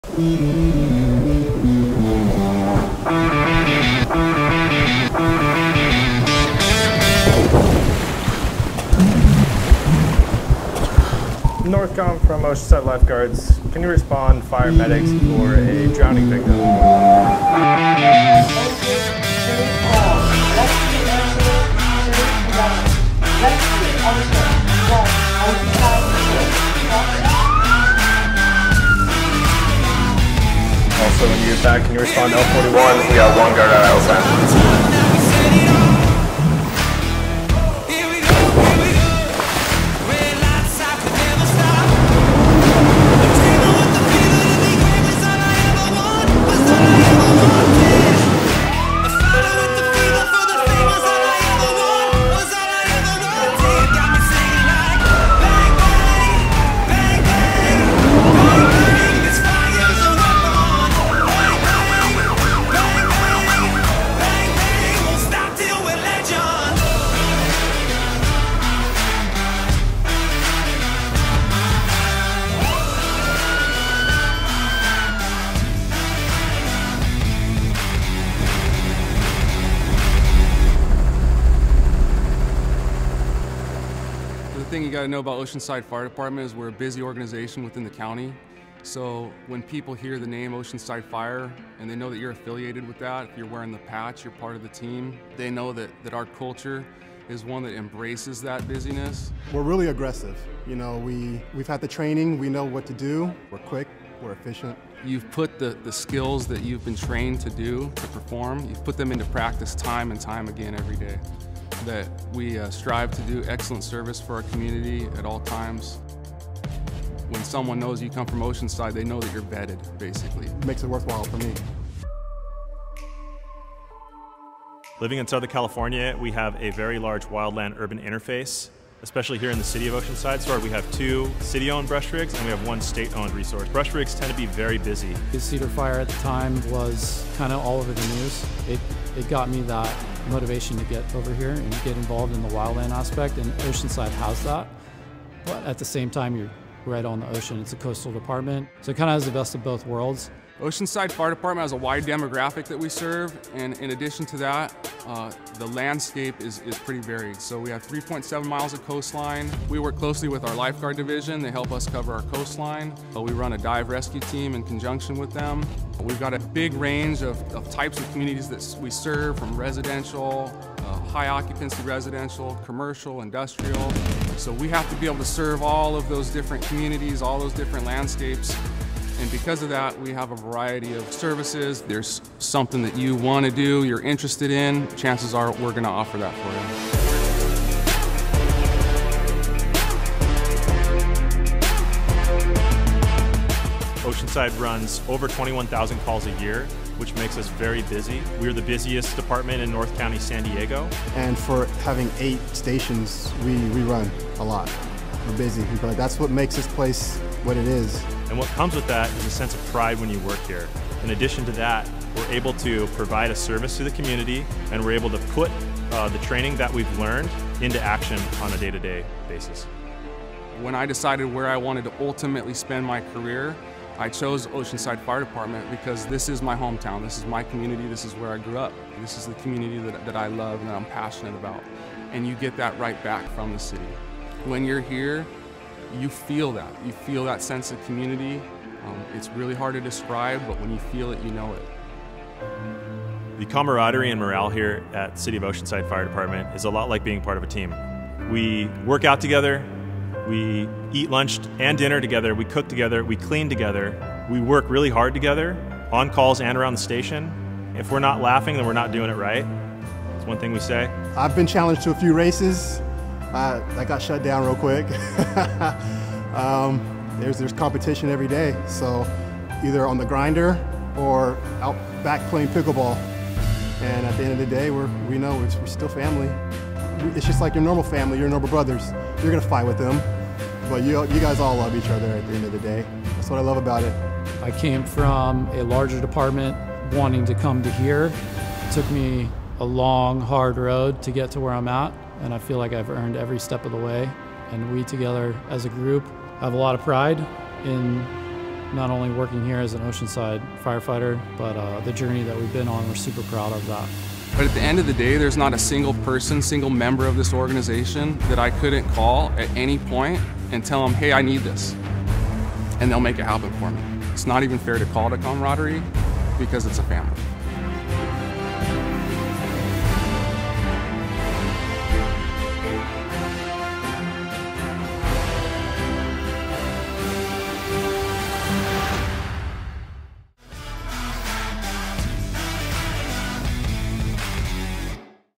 Oceanside Fire Department Recruitment Video
This is who we are and what we are about. If you have the skills, abilities, and a positive attitude, join us and serve a great city!